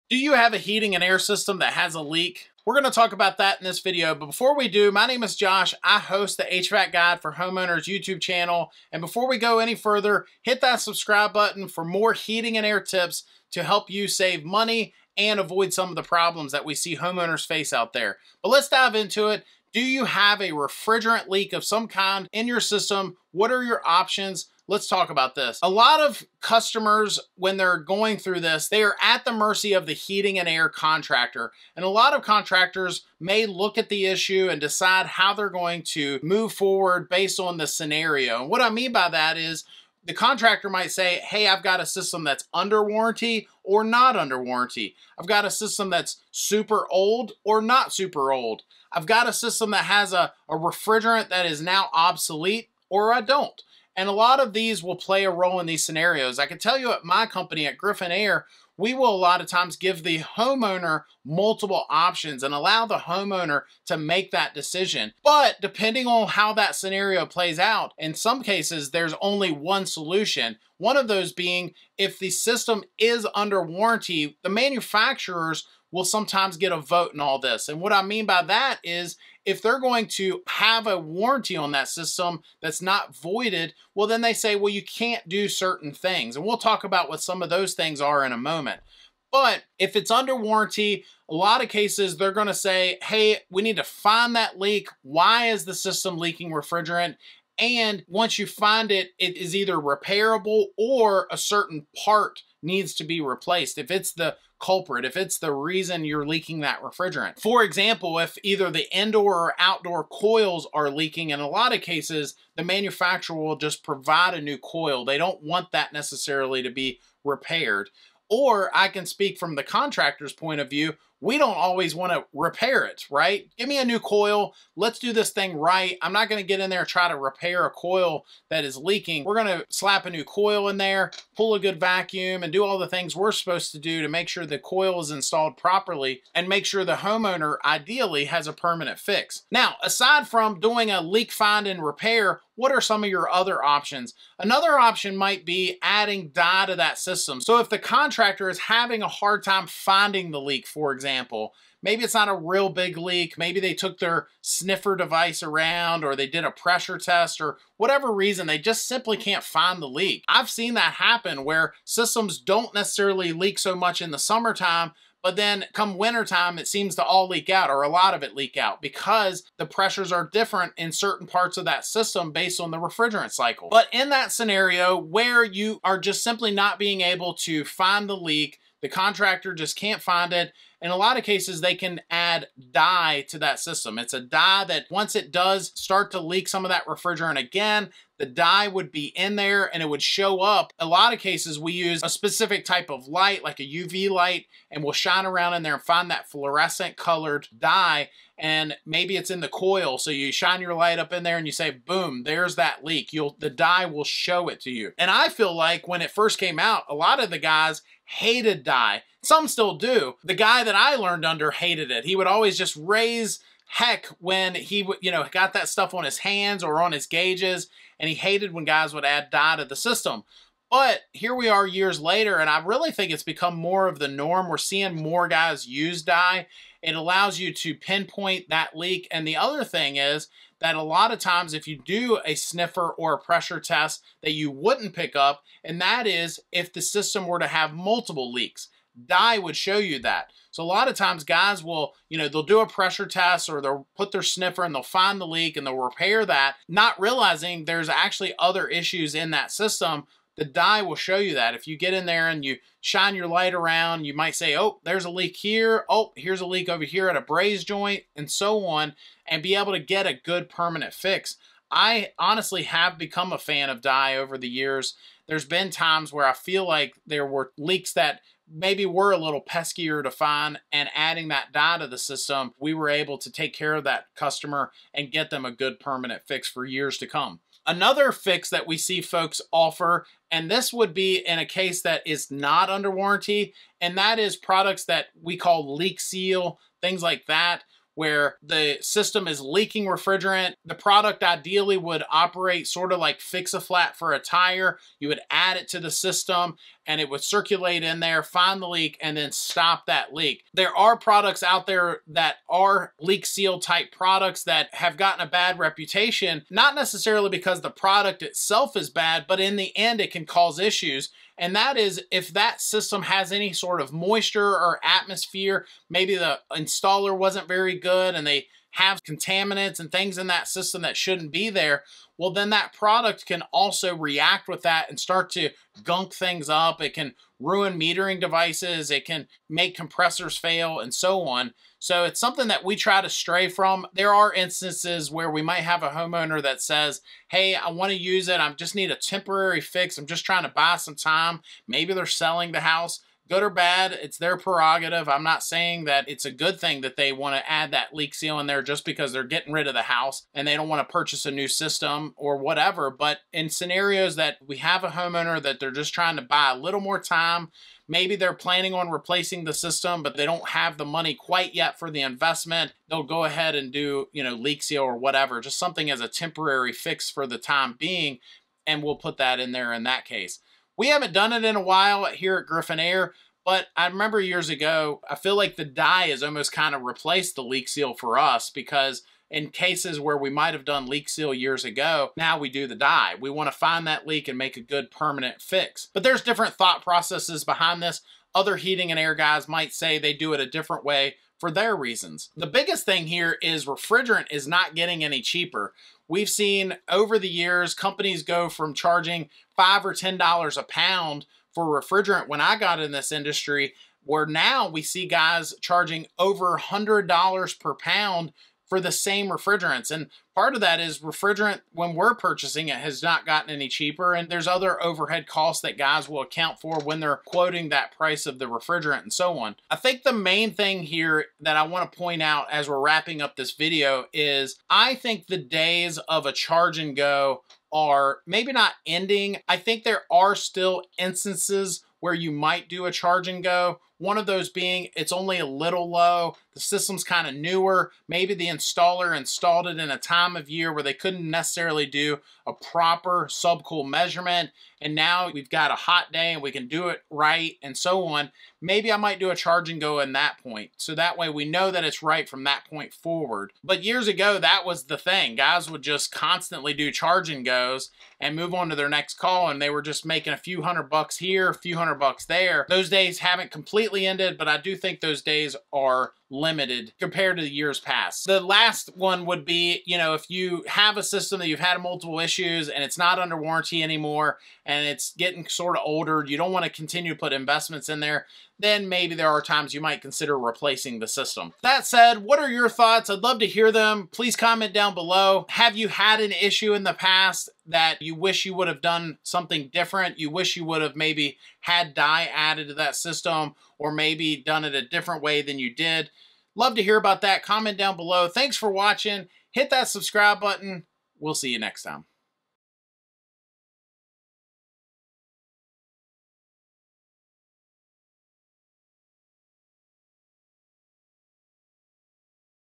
5 Options on AC Refrigerant LEAKS!
This video is about what options are presented to a homeowner and the HVAC technician when they find out the AC system is low on refrigerant. These systems typically have a freon leak and need to be repaired. The reason there are different options and not one goto process for every HVAC system that's low on freon or refrigerant is that systems can be old or new. They can also be under warranty or not. Finally, the leak could be small or huge. These situations are all considered when deciding the best way to move forward for a good permanent fix.

Griffin Air and Joshua Griffin have partnered with Daikin on multiple projects. FTC requires disclosure of these relationships due to the compensation received.

Chapters
0:00 Intro: Refrigerant Leak Options
1:30 Leak Find and Repair
2:24 HVAC Dye
4:37 Leak Seal Products
6:32 Cost of Repairs
7:04 Charge and Go
7:48 Replacement
8:02 Outro